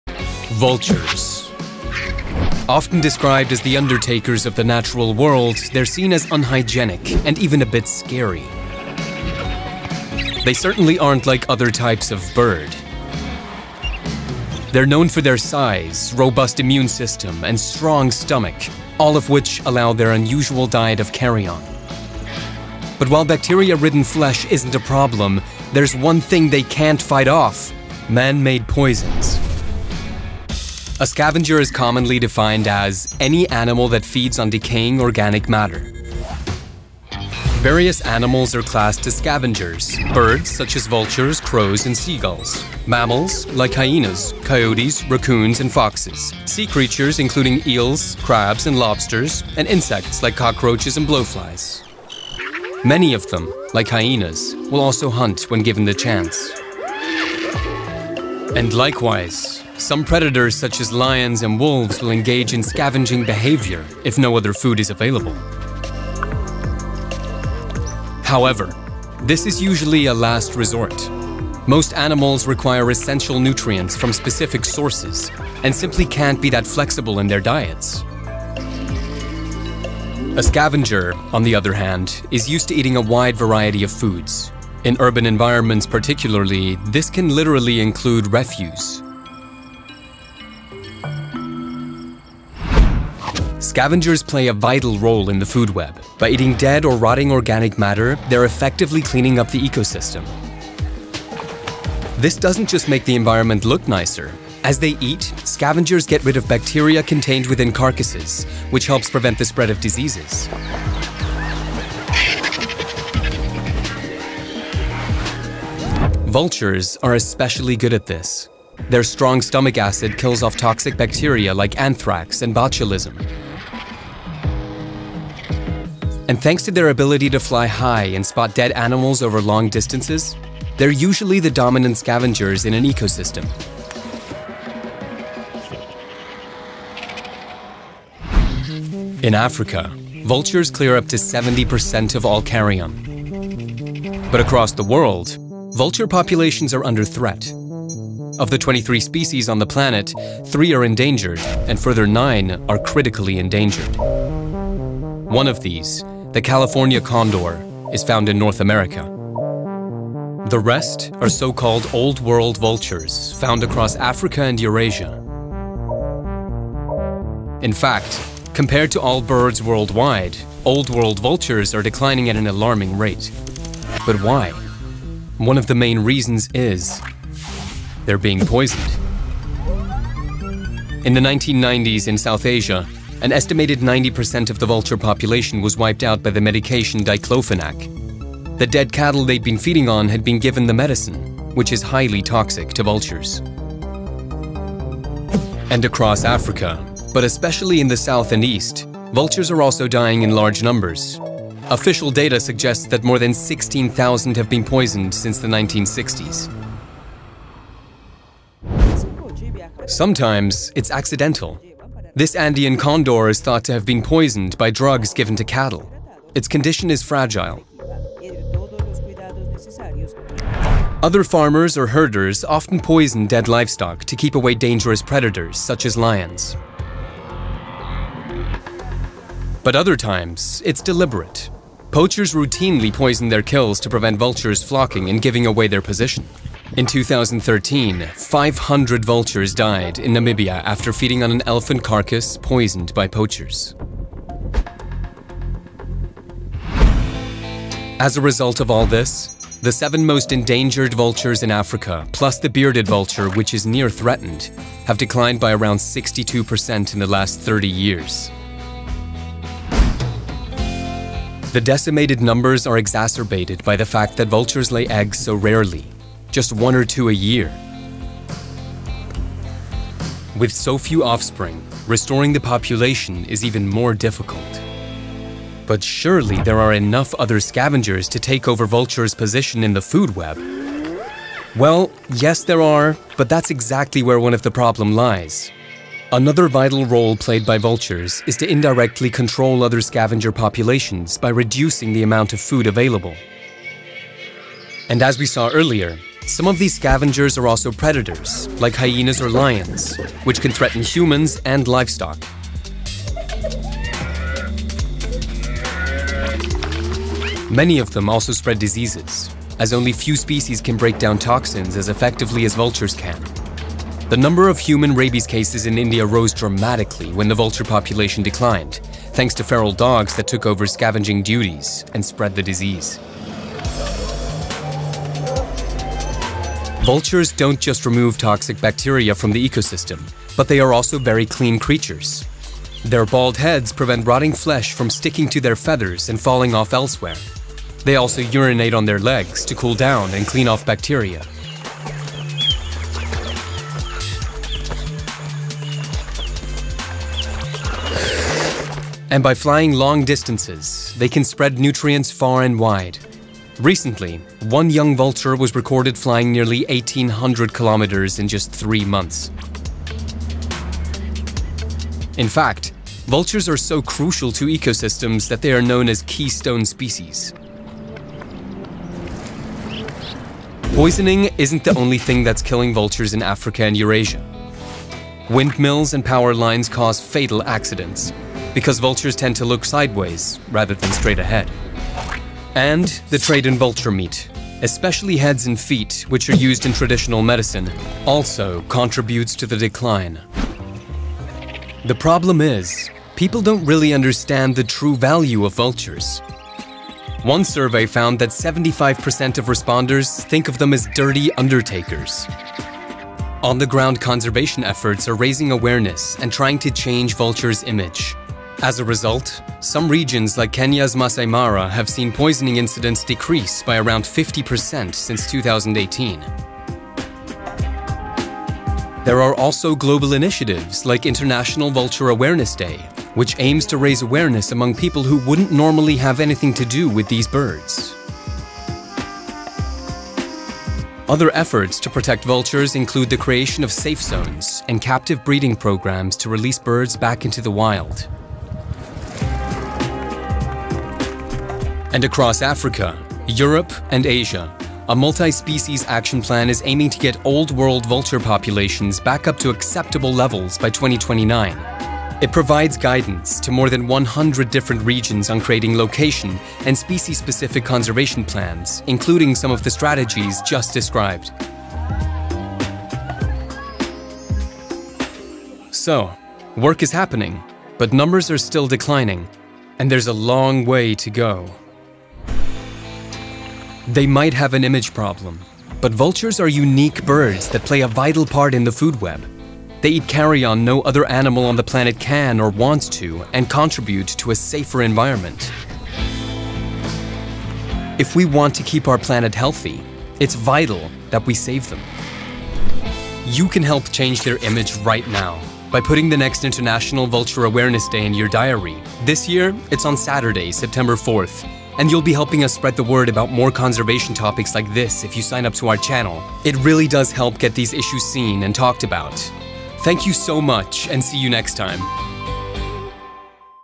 Why our Ecosystem Depends on Vultures I Climate Heroes
Vulture populations across the world are under threat. There are several reasons for their decline – faster than that of all other birds – but chief among them is poisoning by humans. In fact, of the 23 vulture species on the planet, 12 are endangered or critically endangered. But vultures are scavengers, and an essential part of a functioning ecosystem. Their decline can have a huge impact on the environment in ways that might surprise you. Learn how vultures safeguard our health – and what needs to be done to save them.

And please, make a note in your diary: The first Saturday in September each year is International Vulture Awareness Day!

🤓 Fun fact: Vultures are known for vomiting to speedily reduce their weight, particularly when faced with a predator, in order to be able to take-off more efficiently. The practice also has the added benefit of distracting the creature causing the threat, since the chances are that they won't want to look at, or smell, a pile of freshly regurgitated (and often rancid) carrion.

Script: Eleanor Updegraff
Editor: Mathias Muckenhuber
Producer: Eva Schmidt
Graphics: Jörg Eisenprobst
Voice-Over: Julian Nightingall
Audiomix: Hubert Weninger

Footage:
Terra Mater Factual Studios

Production:
Terra Mater Factual Studios GmbH

🔗 Sources:
1. African Wildife Foundation, accessed May 2021,

2. International Vulture Day is Today, Vulture Conservation Foundation, 2 September 2017,

3. The IUCN Red List of Threatened Species 2020: California Condor, IUCN, 28 August 2020,

4. Vultures of Africa and Eurasia, Birdlife International, accessed May 2021,

5. Vulture Conservation: The Case for Urgent Action, Birdlife International, March 2019,

6. Vulture declines: the history, SAVE Vultures, accessed May 2021,

7. African Wildlife Poisoning Database via Vulpro, South Africa, November 2019,

8. Another Continental Vulture Crisis: Africa's Vultures Collapsing Towards Extinction, Darcy Ogada et al, 18 June 2015,

9. What's Killing Africa's Vultures, Birdlife International, accessed May 2021,